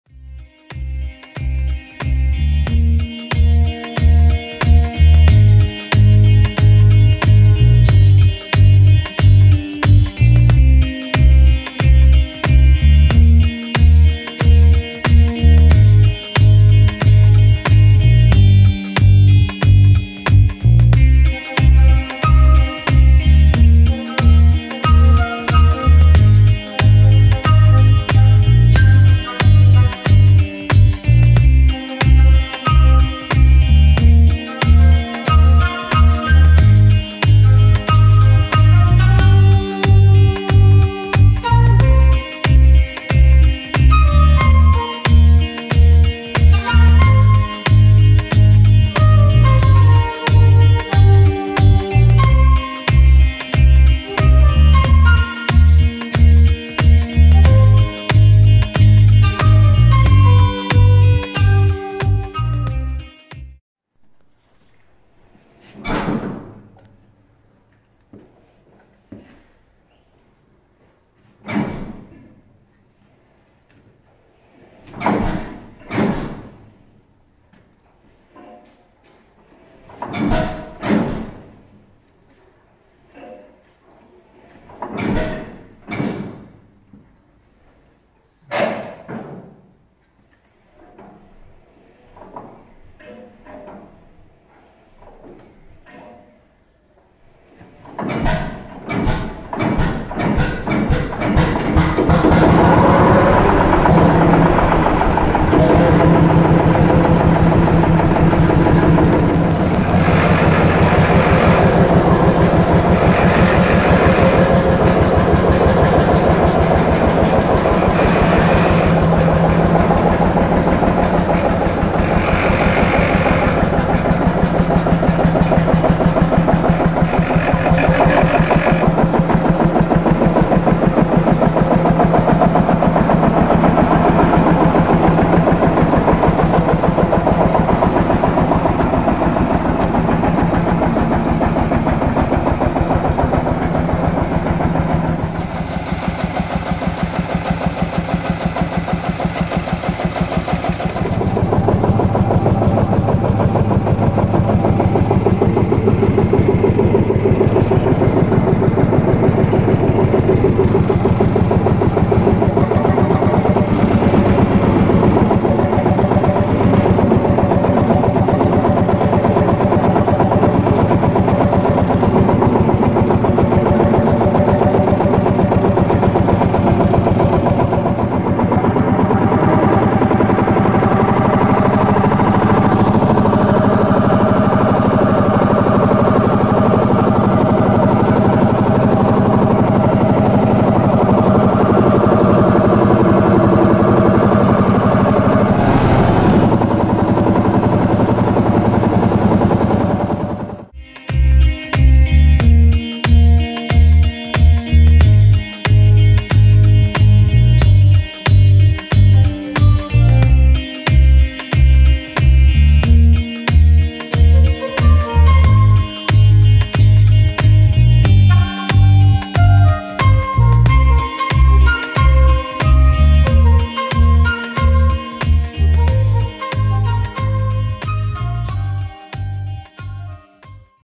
Lanz Bulldog: Die Restauration - Erster Start nach 50 Jahren
Hier das Video zum ersten Start am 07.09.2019.

Bis dato stecken vom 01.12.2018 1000 Arbeitsstunden in dem Projekt. Fertig ist es allerdings noch nicht ;)
Die anderen Videos werden natürlich im Laufe der nächsten Monate folgen!